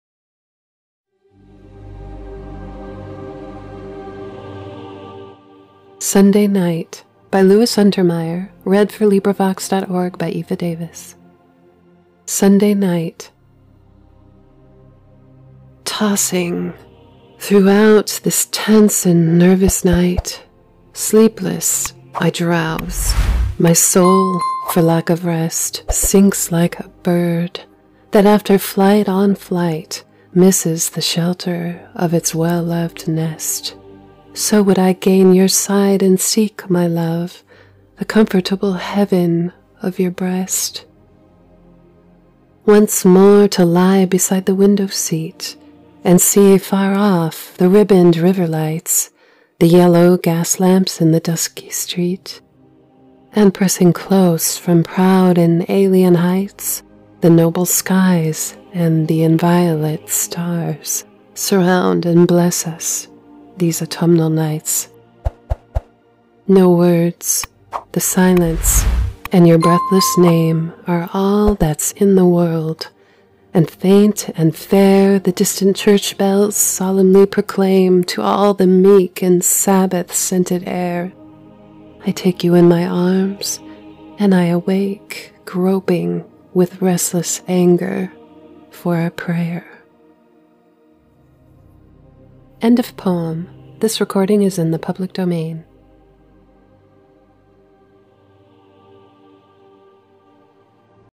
Sunday Night. Audio book. Challenge. Motivation Video #2021
LibriVox recording of Challenge by Louis Untermeyer.
Read in English by Larry Wilson; aniroo; Bill Mosley; Mark Warner; Eva Davis; Ken Davies; Wolfgang Bas; DrPGould; Nemo
Louis Untermeyer introduced may students to poetry through his many collections that he edited. His own poetry ranges from inspirational to cynical. Challenge is his first collection of poems, mature, but reflecting many of his early struggles. The range from the deeply spiritual to the sentimental and social commentary. - Summary by Larry Wilson

For more free audio books or to become a volunteer reader, visit LibriVox.org